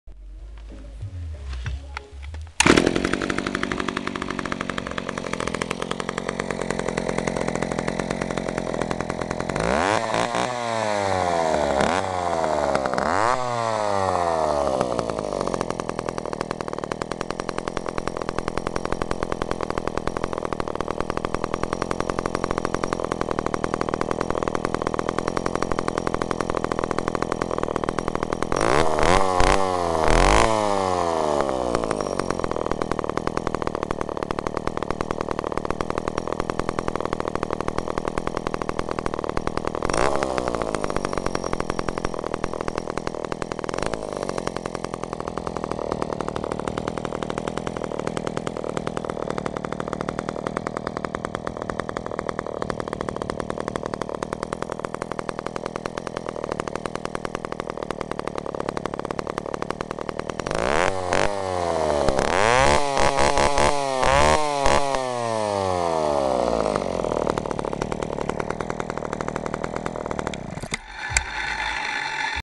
Rare John Deere 30 Chainsaw Test Run Echo CS-302
Older John Deere 30 chainsaw , start up and runs perfect, chain oiler with oil amount adjuster works well. Very nice little saw in excellent shape.